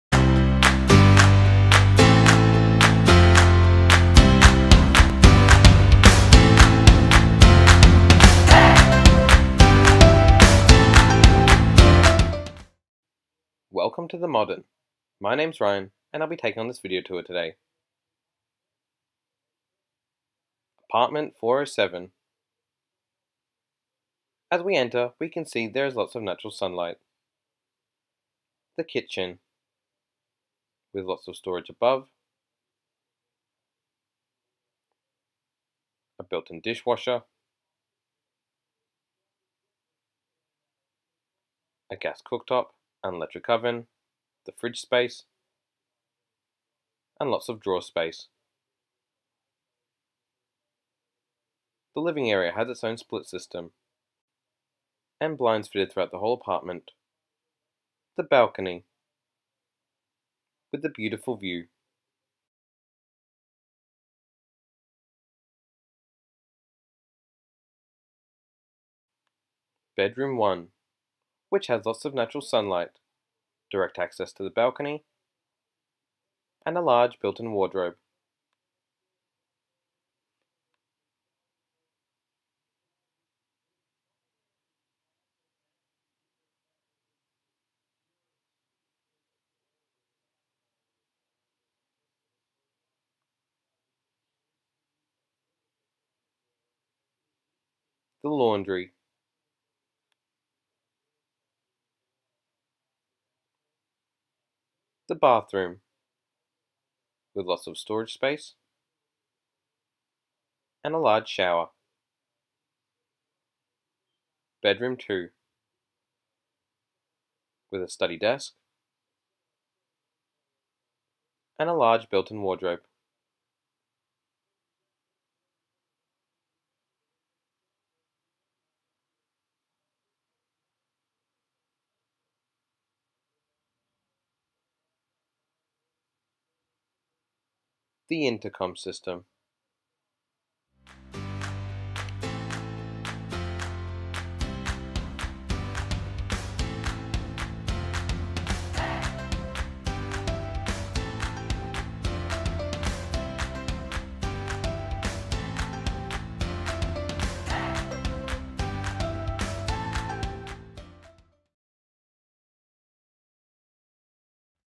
Apartment 407/501 Plenty Road, Preston
Come and view our video tour of an amazing 2 bedroom apartment!

Don't miss out on this amazing opportunity to live within one of Melbourne's most desirable suburbs.

Make your way into your new home now!